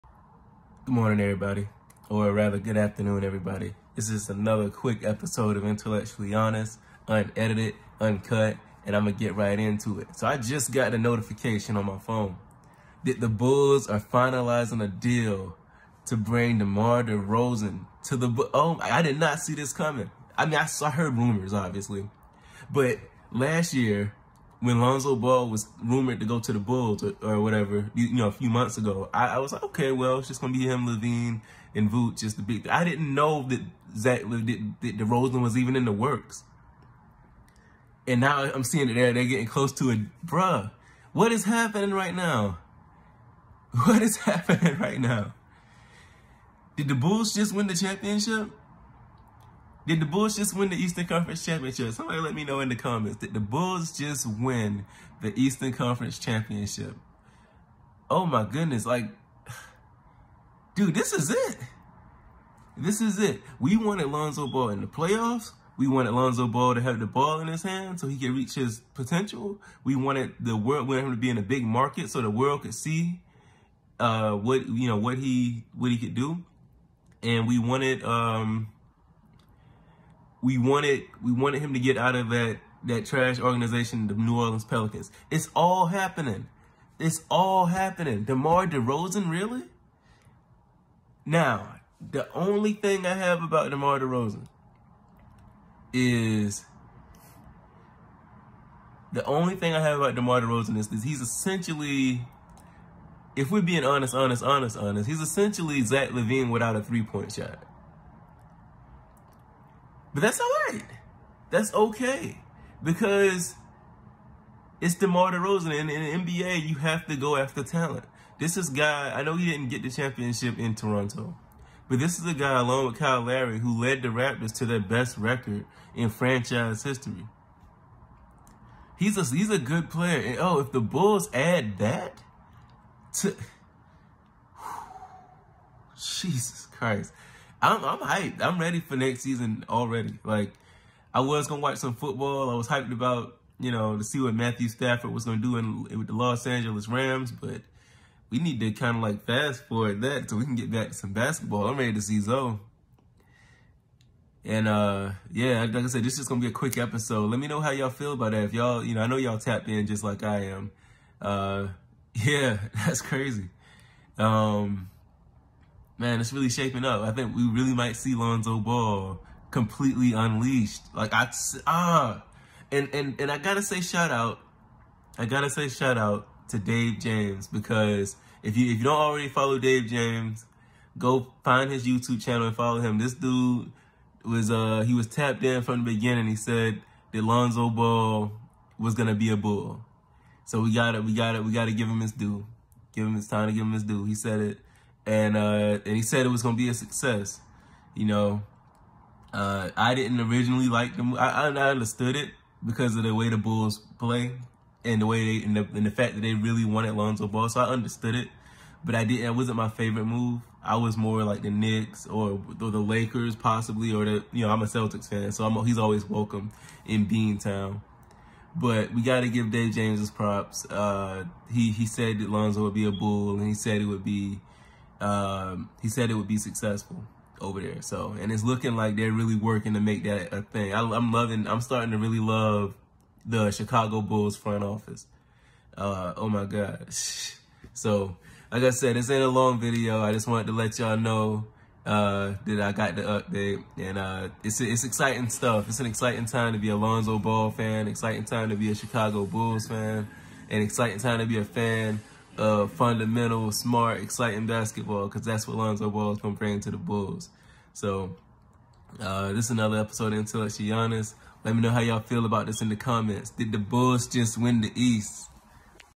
Did the Chicago Bulls just win the Eastern Conference??
Reacting to DeMar Derozen close to signing with the Bulls